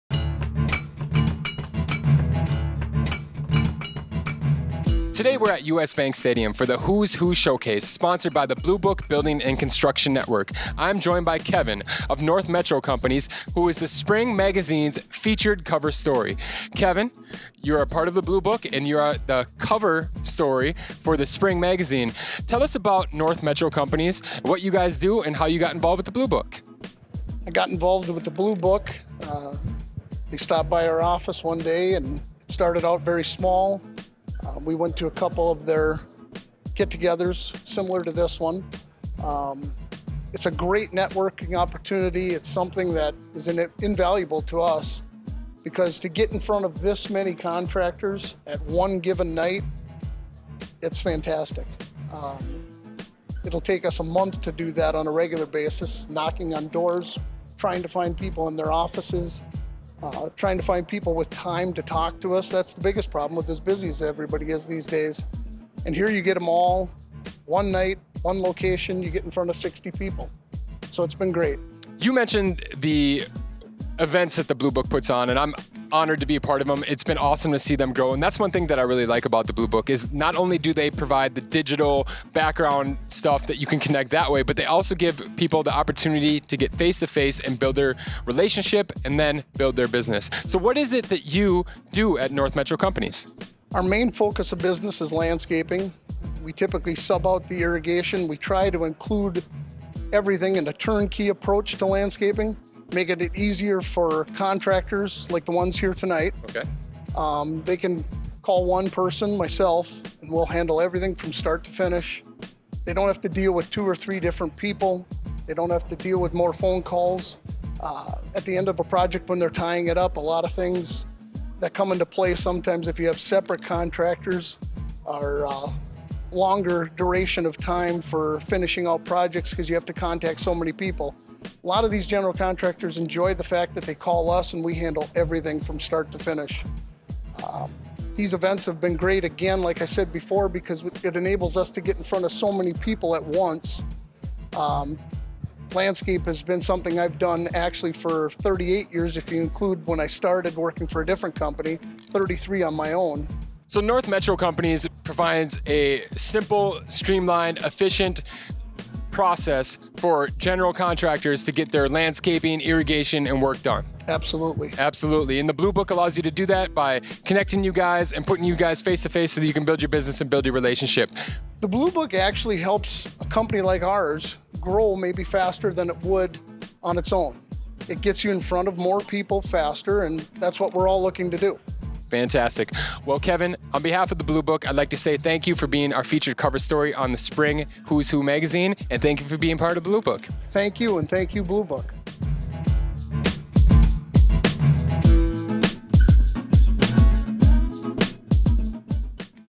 North Metro Companies Reviews The Who's Who Showcase
Kevin Terhaar of North Metro Companies reviews The Who's Who Showcase held at U.S. Bank Stadium in Minneapolis, MN on March 28, 2018.